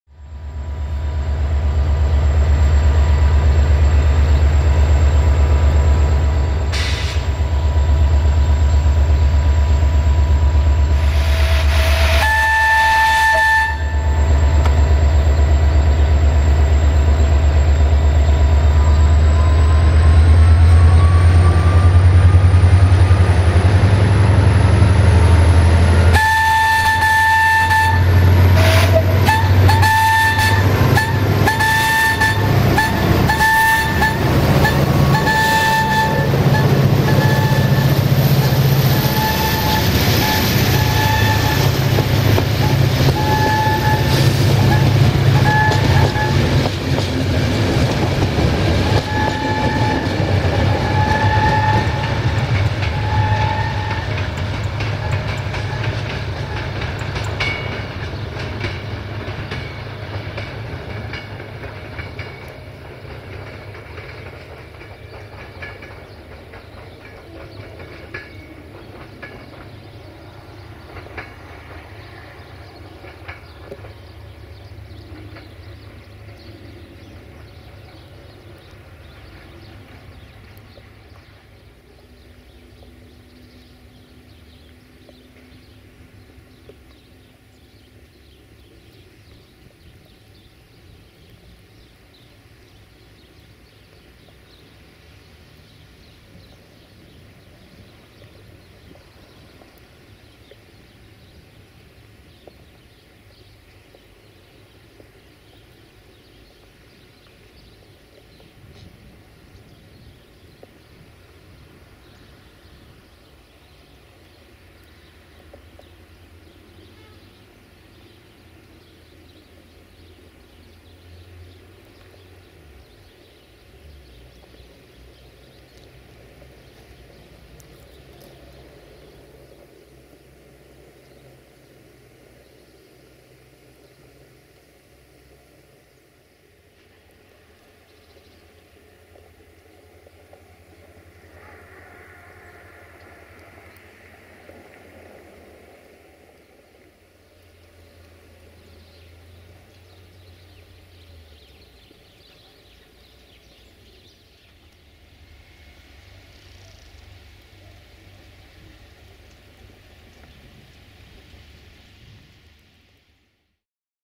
Jimmy 65-1300-6 & Duewag BR628 654-5 in Gara Bratca Station [Winter Edition] - 13 January 2018
1:IR367-2"Harghita" - Budapesta Keleti - Braosv - Regina Bihorului~Jimmy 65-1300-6 RO-SNTFC
2:IR15855 - Cluj Napoca - Oradea - Automotor Duewag~BR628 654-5 IRC/DB Regio

[Winter Edition]

13 January 2018